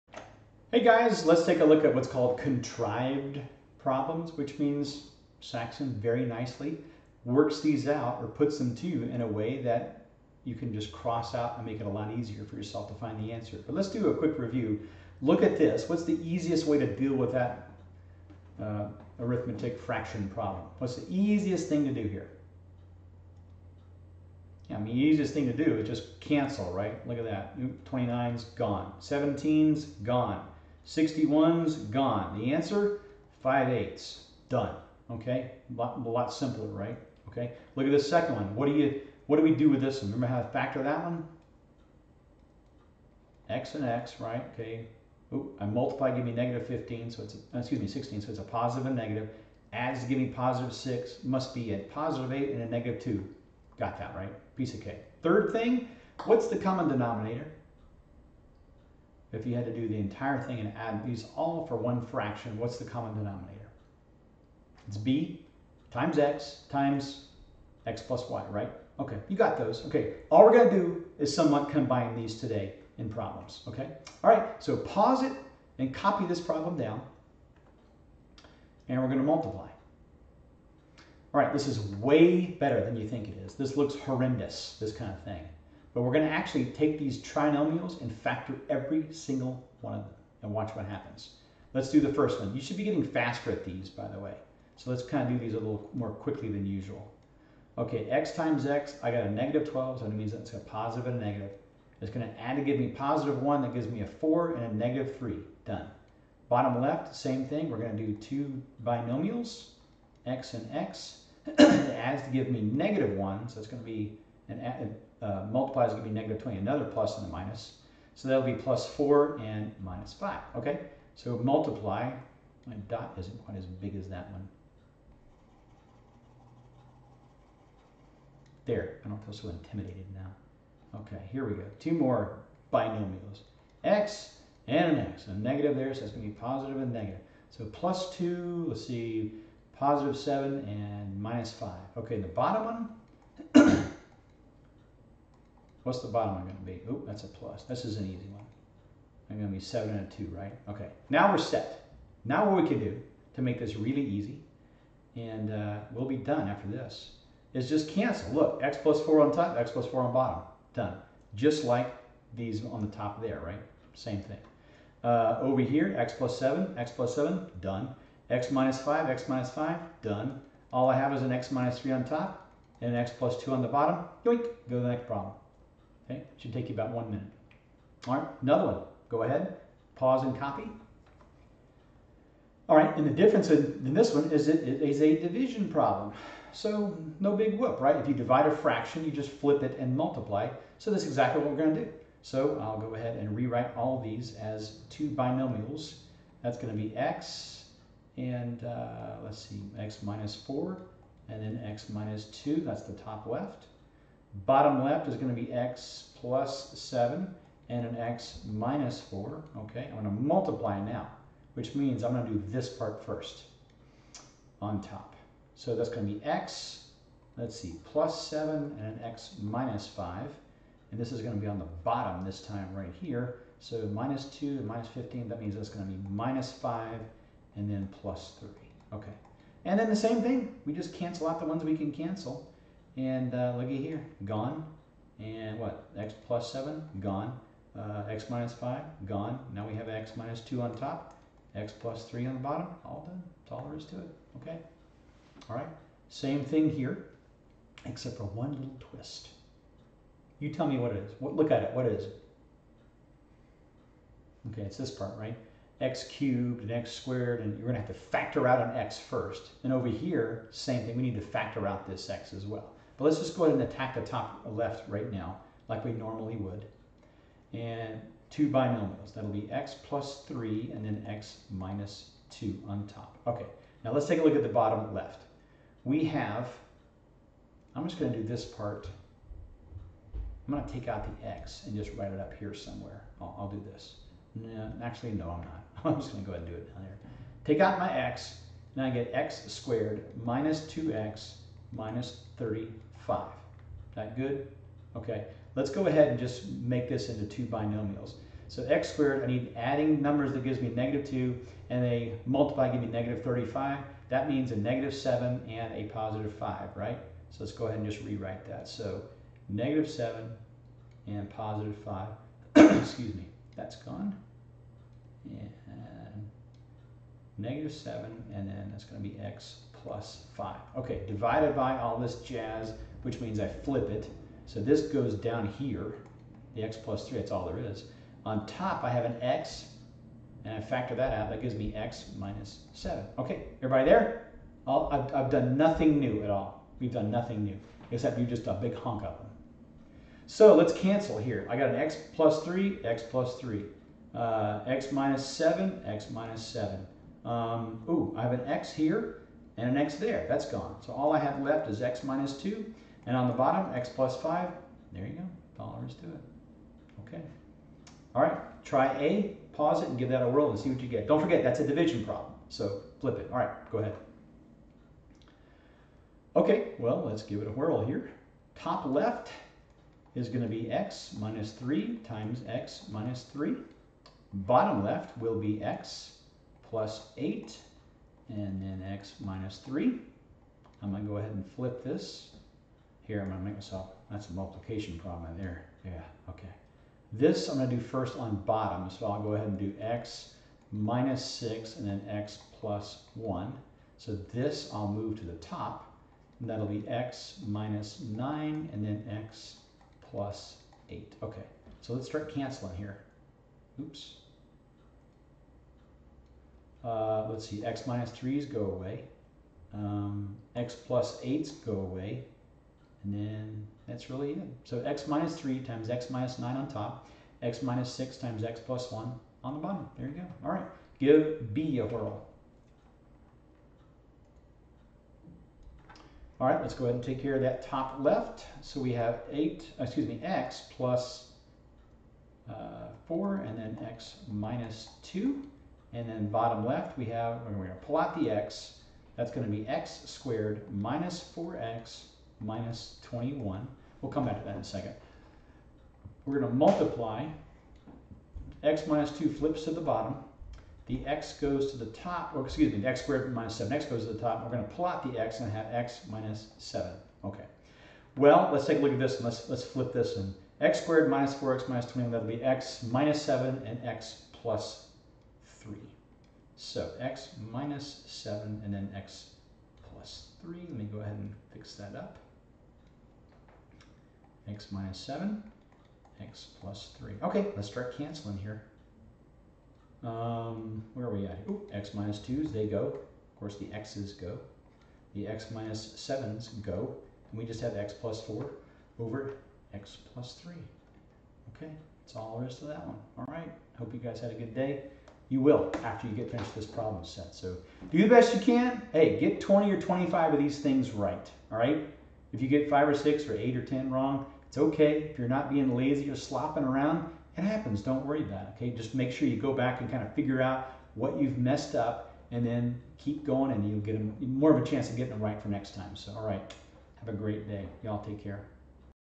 Saxon Math - Algebra 2: 3rd Ed. (Lesson 36 - Mult. and Div. Rational Expressions) - SEE CORRECTION !
Enjoy this free video lesson for the great John Saxon's Algebra 2 3rd Edition textbook.  (This set only goes through Lesson 114 of 129, just FYI.)